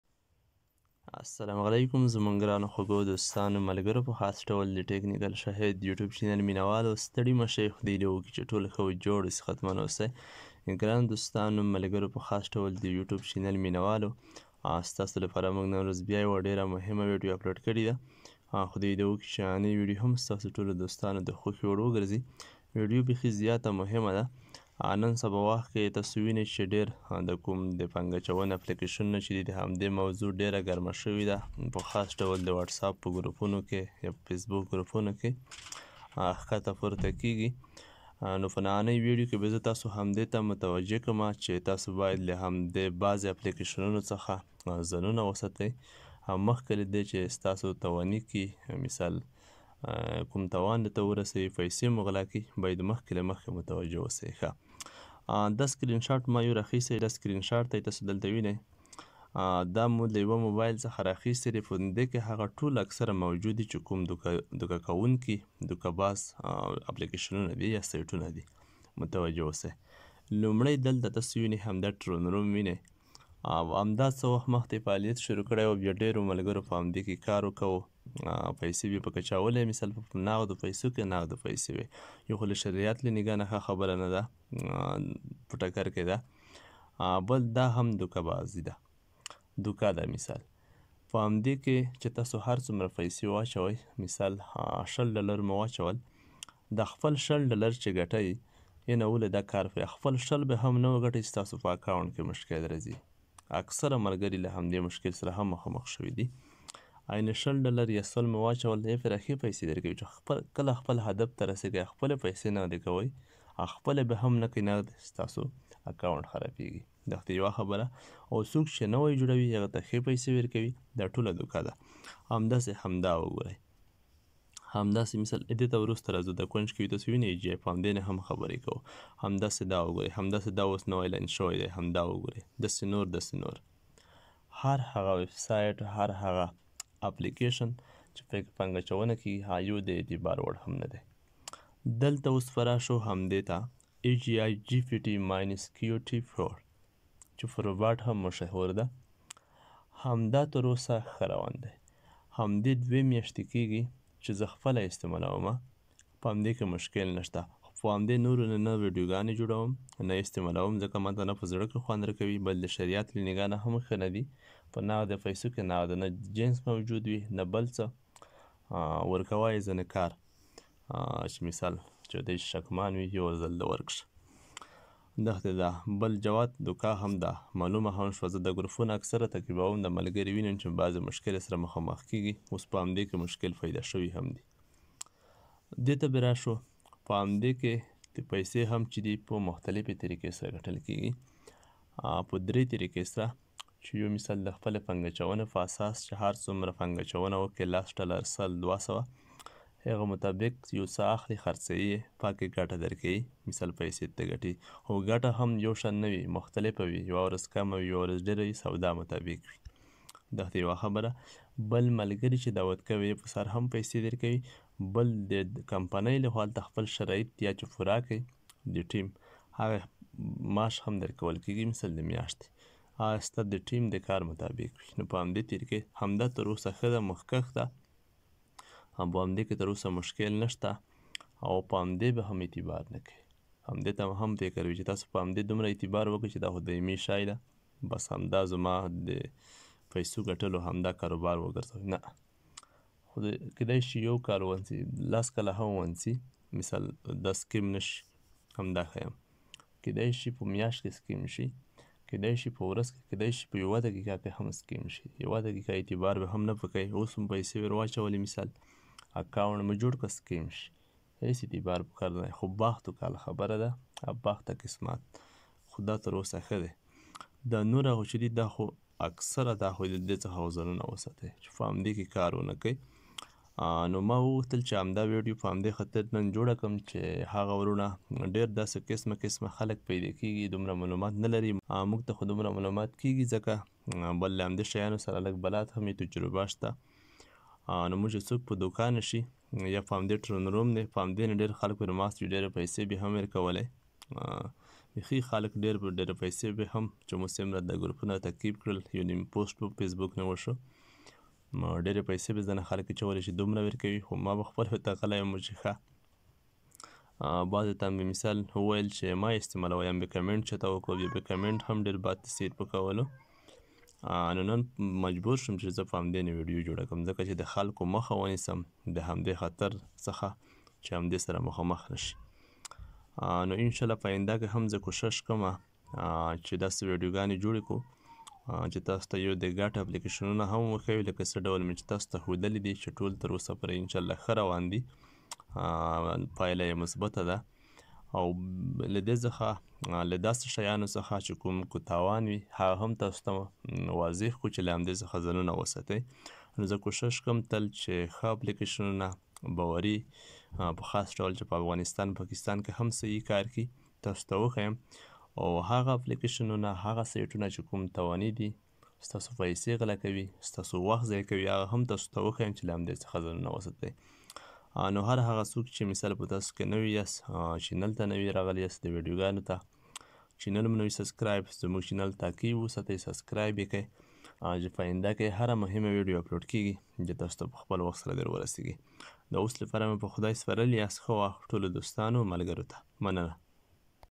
د انلاین پیسو ګټلو خطرناک اپلېکېشنونه او سایټونه وپیژنئ
جالي افلیکیشنونو او سایټونو پیژندل